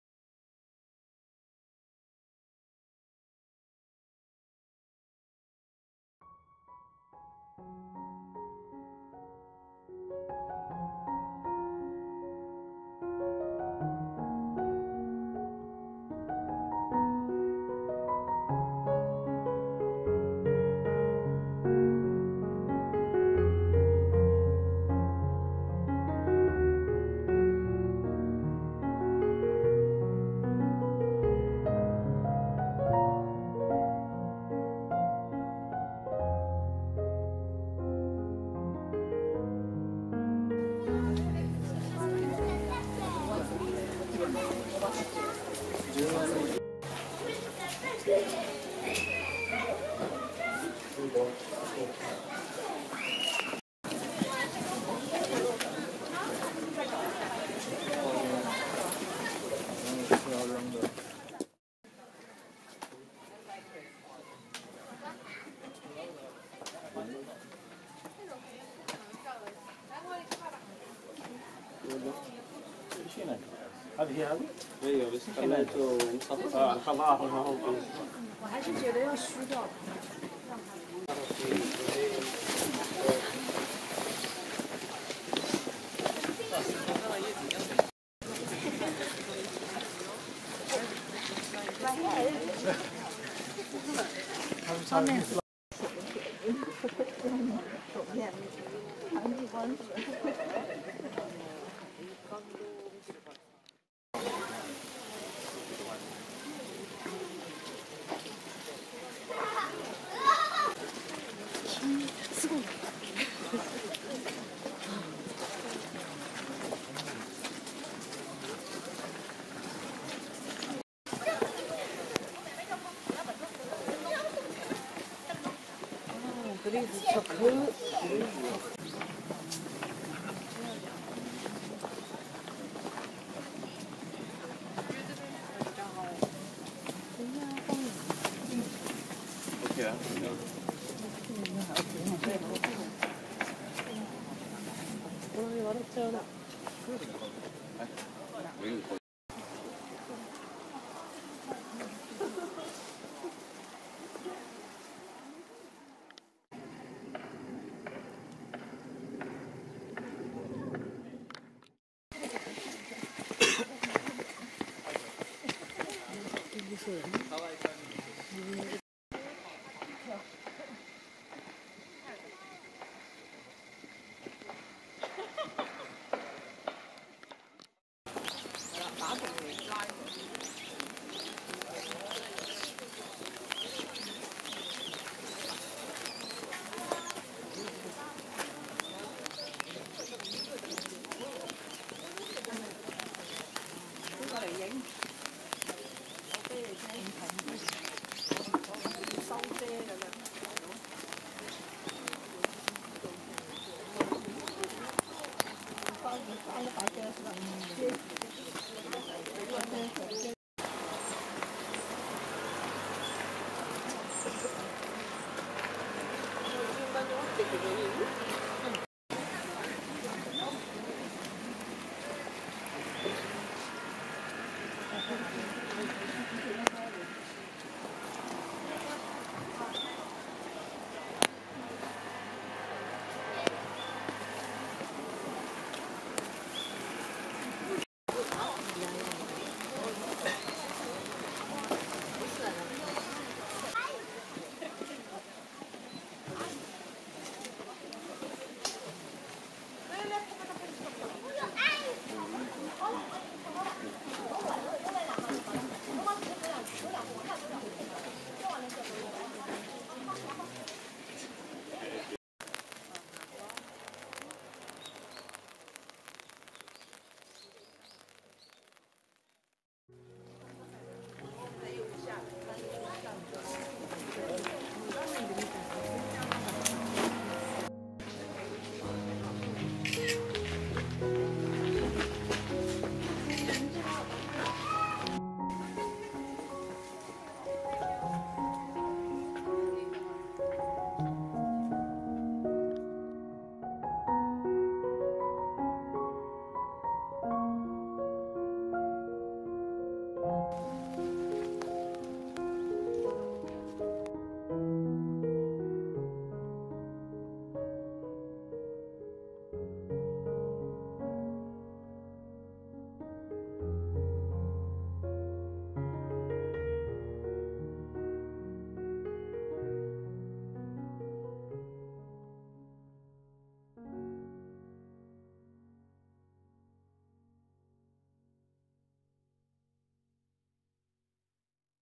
常寂光寺の紅葉 Jojakkoji temple in Kyoto 2019年11月28日
阪急嵐山駅から混雑と小雨の中を常寂光寺に行きました。
人混みの中を、傘をさしての撮影は、結構大変ですが、雨に濡れた紅葉も、また、いいのではと思います。